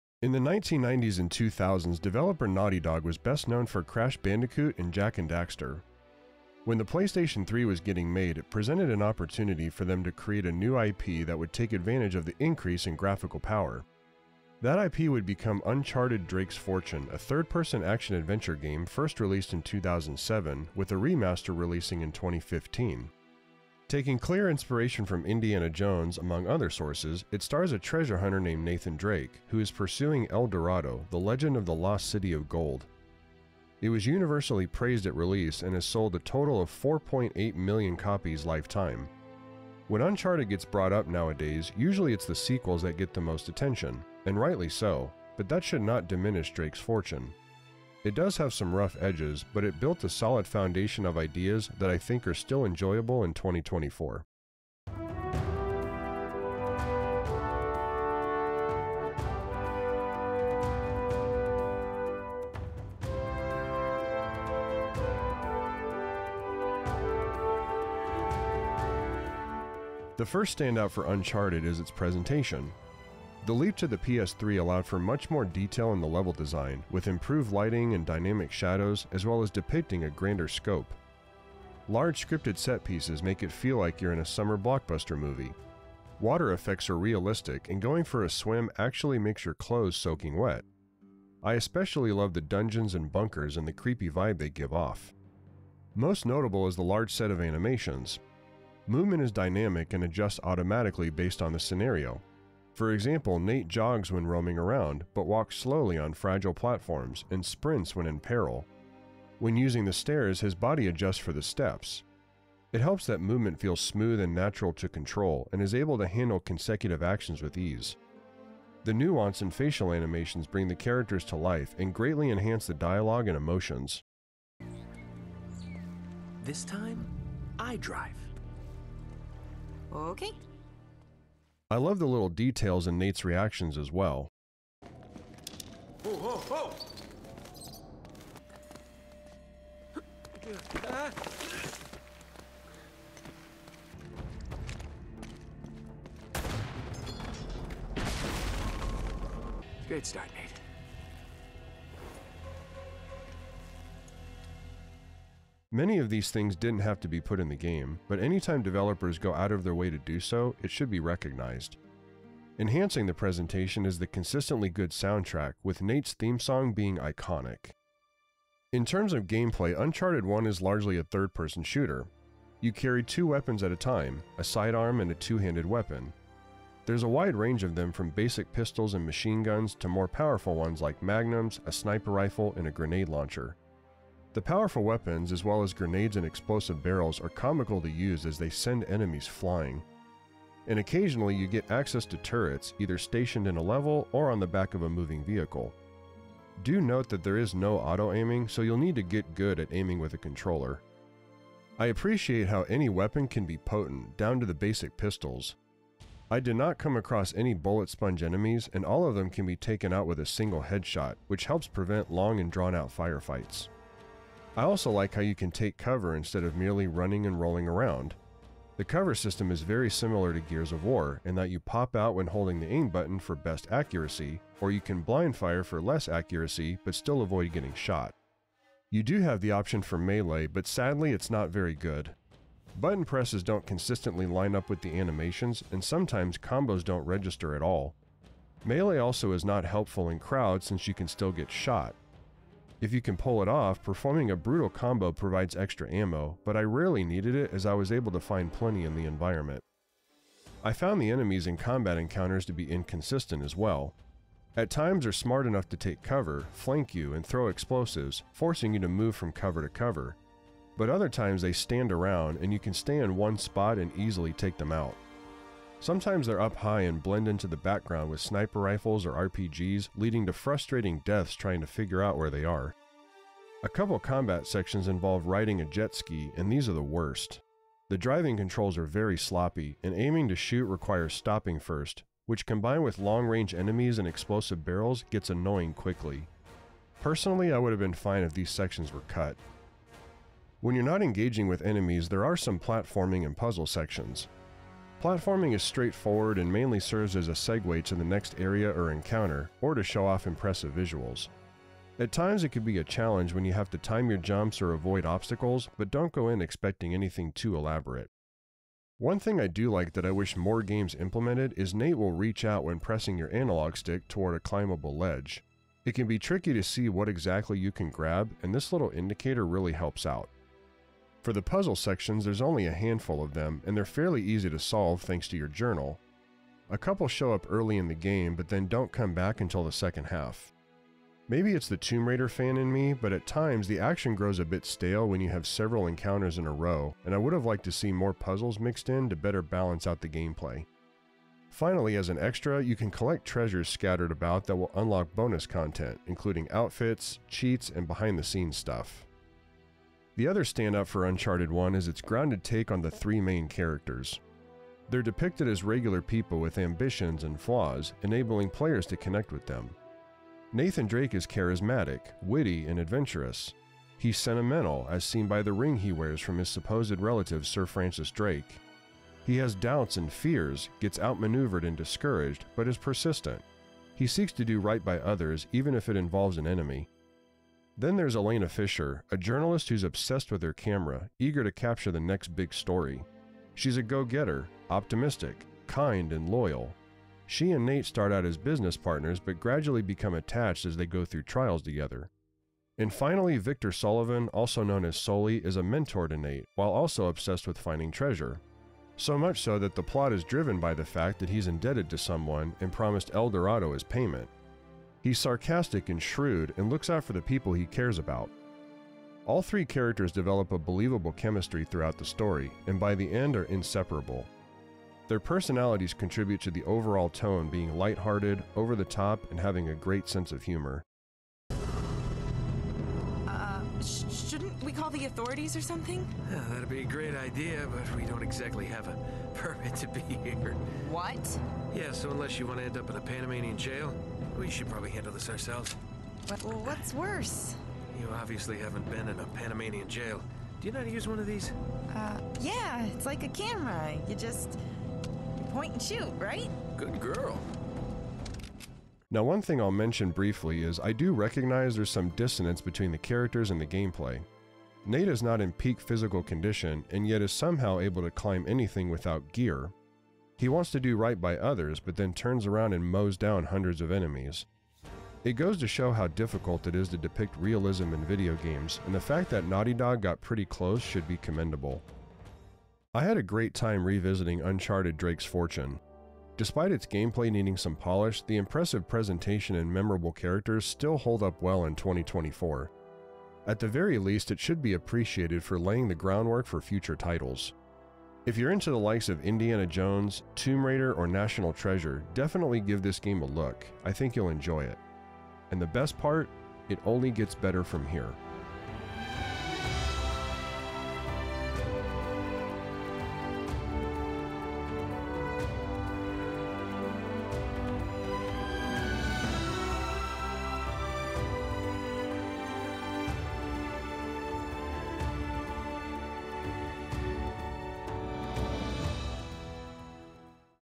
Revisiting Uncharted: Drake's Fortune (2024 Review)
When Uncharted gets brought up nowadays, usually it's the sequels that get the most attention, and rightly so, but that should not diminish Drake's Fortune.  It does have some rough edges, but it built a solid foundation of ideas that I think are still enjoyable in 2024.